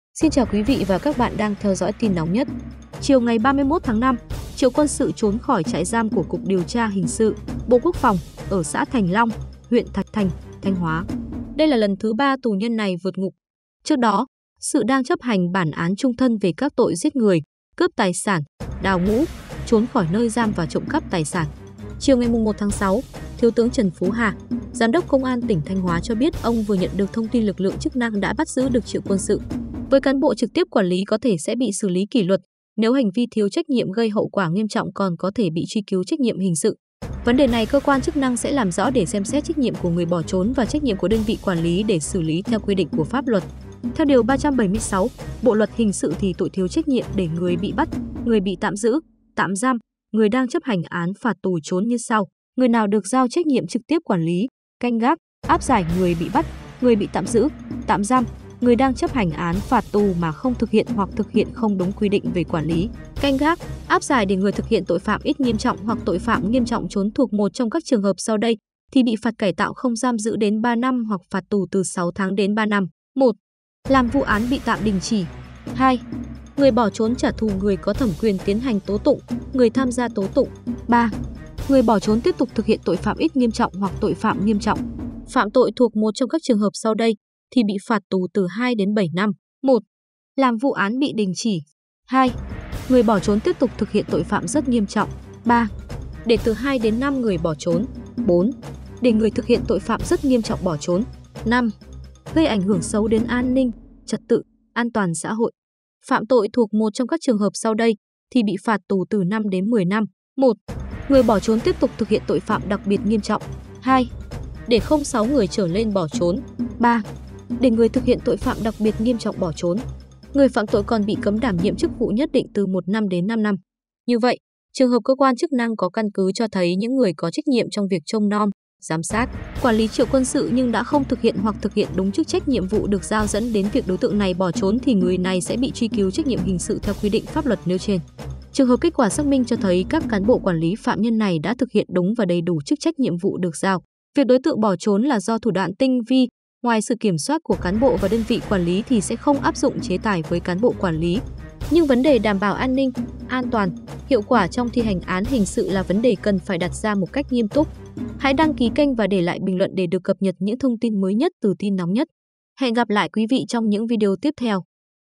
Cán bộ trông coi Triệu Quân Sự có bị truy cứu trách nhiệm hình sự không ? | TIN NÓNG NHẤT
Chiều 31/5, Triệu Quân Sự trốn khỏi trại giam của Cục Điều tra hình sự (Bộ Quốc phòng) ở xã Thành Long, huyện Thạch Thành, Thanh Hóa. Đây là lần thứ 3 tù nhân này vượt ngục. Trước đó, Sự đang chấp hành bản án chung thân về các tội Giết người, Cướp tài sản, Đào ngũ, Trốn khỏi nơi giam và Trộm cắp tài sản.
Chiều 1/6, thiếu tướng Trần Phú Hà, Giám đốc Công an tỉnh Thanh Hóa cho biết ông vừa nhận được thông tin lực lượng chức năng đã bắt giữ được Triệu Quân Sự.
Với cán bộ trực tiếp quản lý có thể sẽ bị xử lý kỷ luật, nếu hành vi thiếu trách nhiệm gây hậu quả nghiêm trọng còn có thể bị truy cứu trách nhiệm hình sự. Vấn đề này cơ quan chức năng sẽ làm rõ để xem xét trách nhiệm của người bỏ trốn và trách nhiệm của đơn vị quản lý để xử lý theo quy định của pháp luật.

TIN NÓNG NHẤT là kênh cung cấp thông tin nhanh, chuẩn xác và hữu ích hàng đầu.